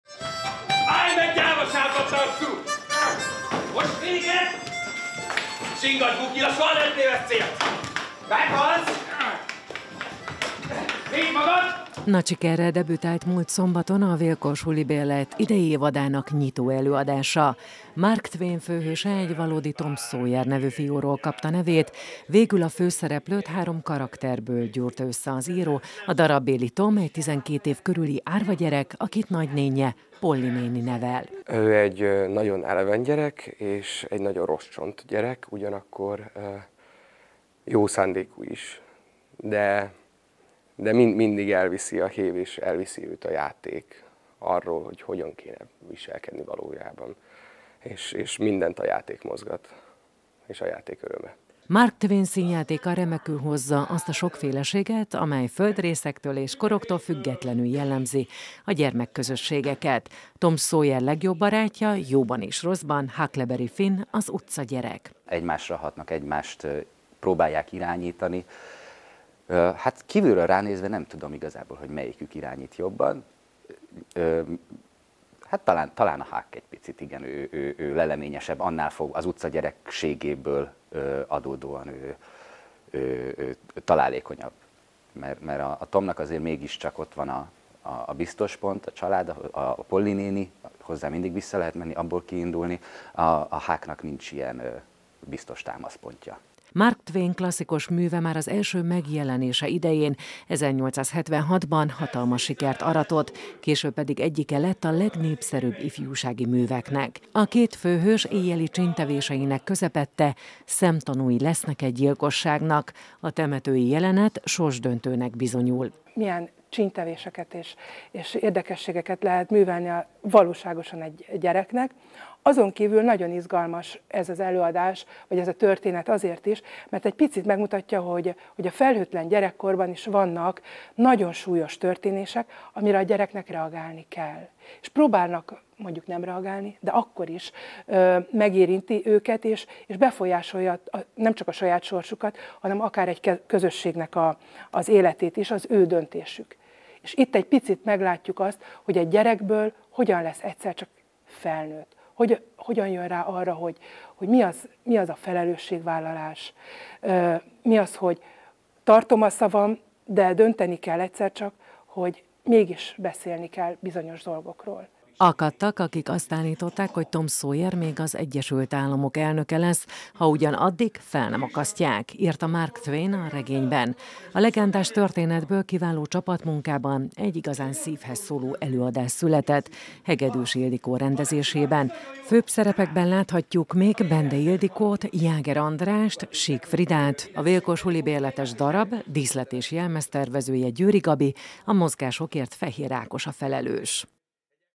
Tom Sawyer kalandjai
Múlt szombaton nagy sikerrel debütált az Vill-Korr Sulibérlet idei évadának nyitó előadása. A Hegedűs Ildikó rendezte Tom Sawyer kalandjai című ifjúsági színjáték eredeti premierjét korábban, 2020 decemberében maszkban és az online térben tartották, most azonban végre a nagyközönség is láthatja a szívhez szóló előadást a Kisfaludy Teremben. A humoros és drámai történet visszavezet bennünket a gyermekkorunkba, a két főszereplőt Karácsony Gergely és Kurucz Dániel alakítják.